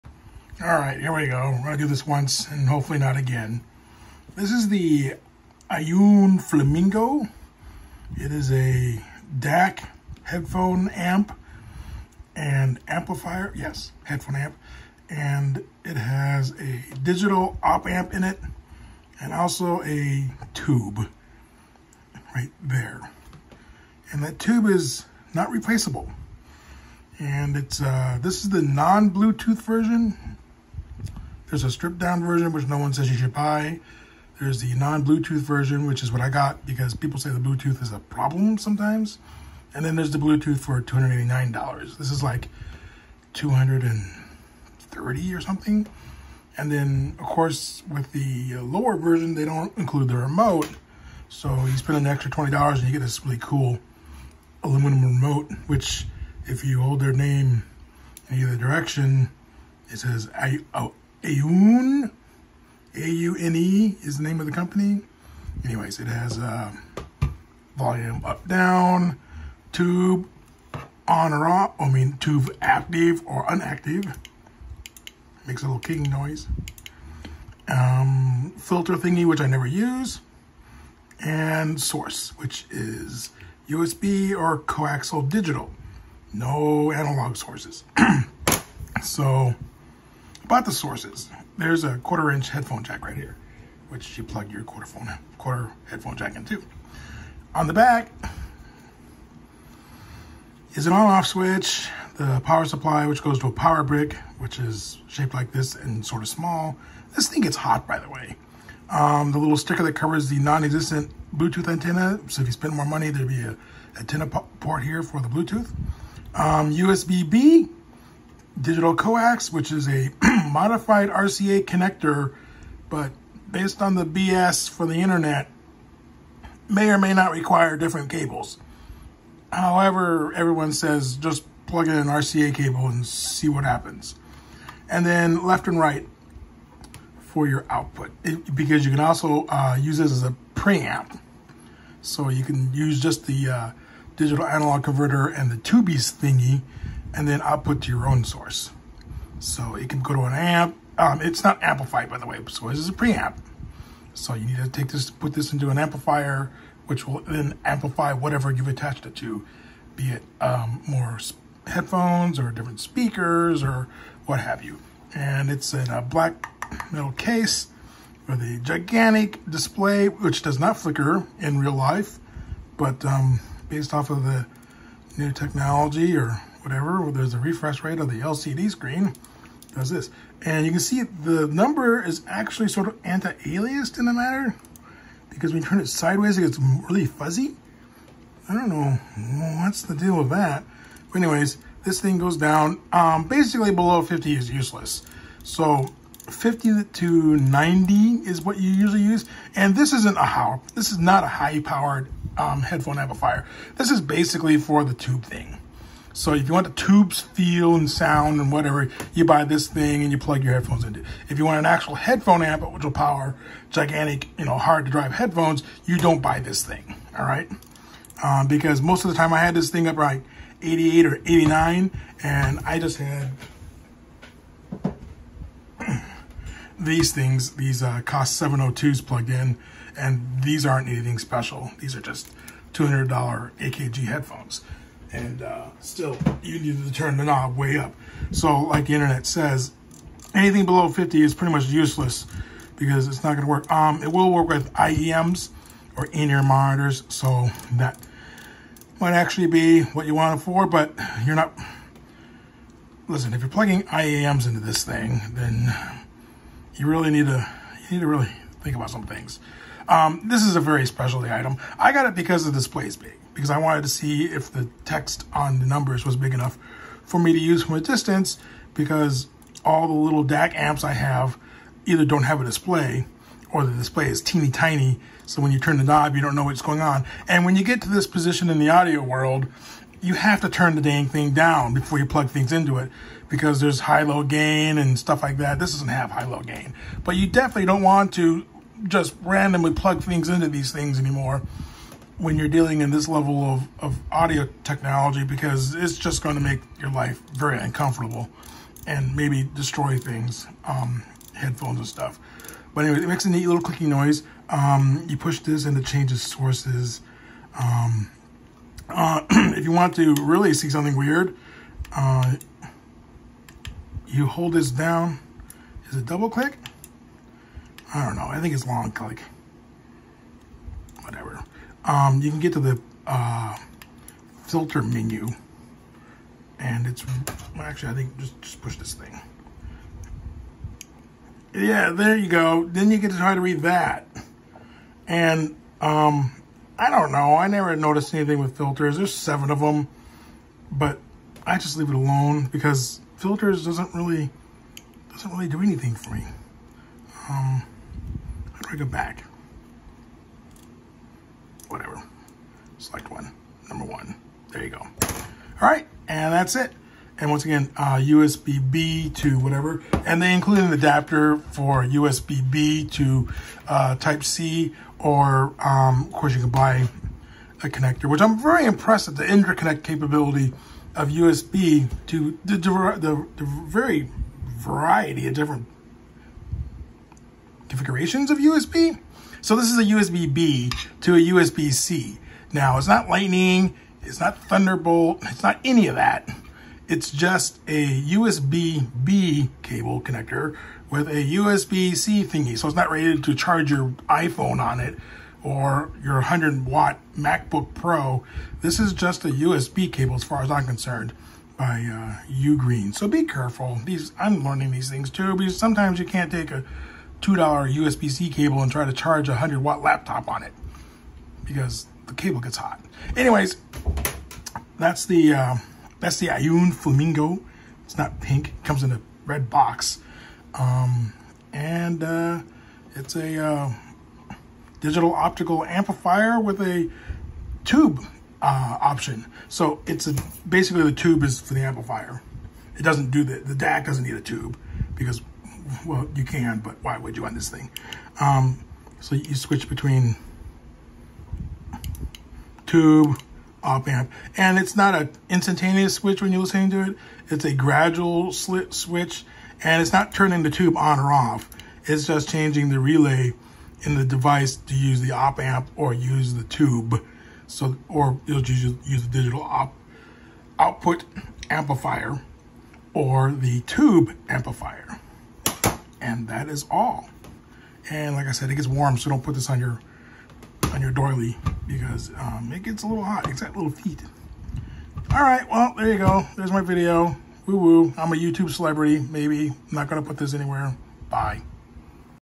Aune Flamingo DAC/ headphone Amp with tube.
Down and dirty video about the Aune Flamingo Headphone DAC/Amp with a tube.